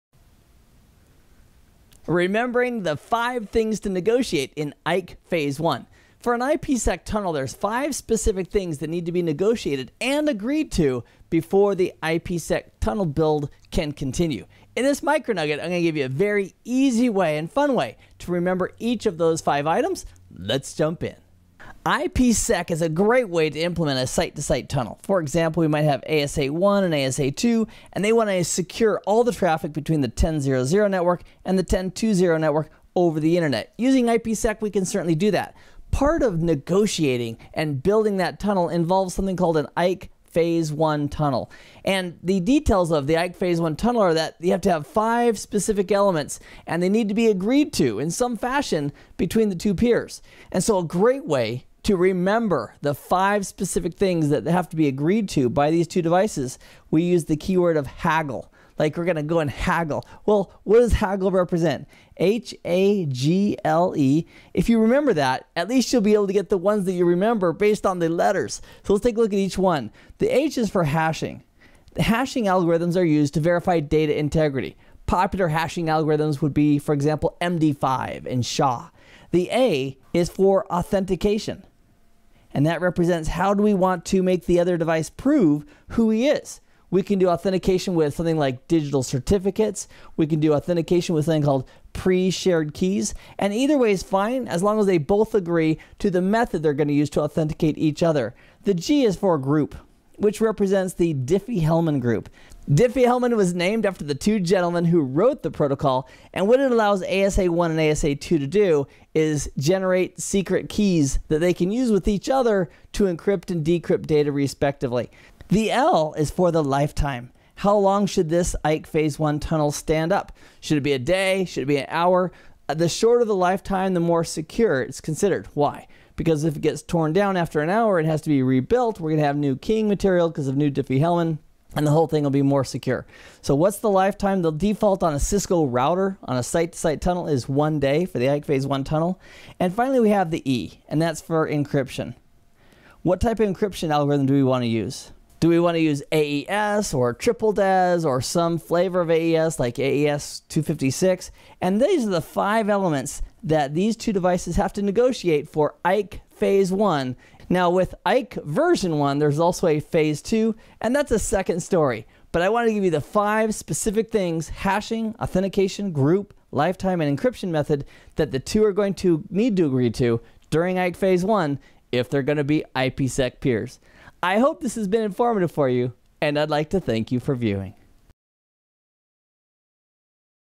MicroNugget Remembering the 5 Things to Negotiate in IKE Phase 1 (IPsec)
In this MicroNugget, I'll provide an easy and fun way for remembering 5 specific items needed for building an IPsec tunnel.